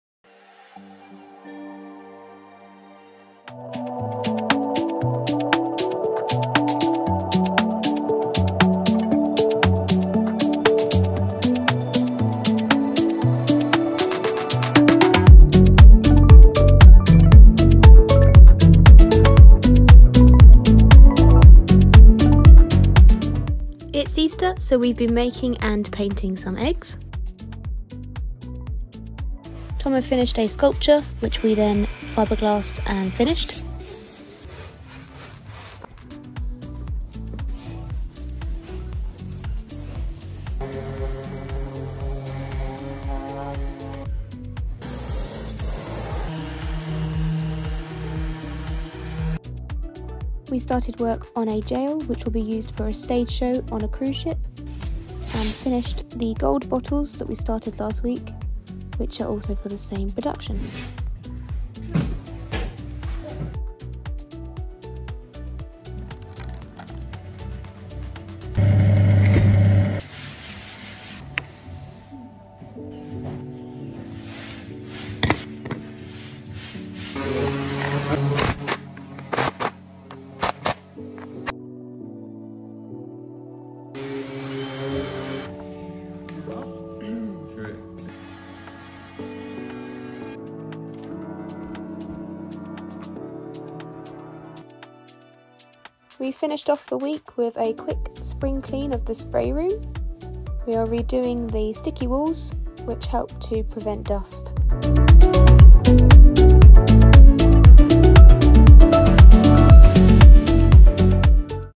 Spur Diaries (31/03/24)
Spur Diaries week 12 (31/03/24)
Happy Easter, we’ve been making eggs, fibreglassing a large sculpture, and finishing a jail for a cruise ship stage show.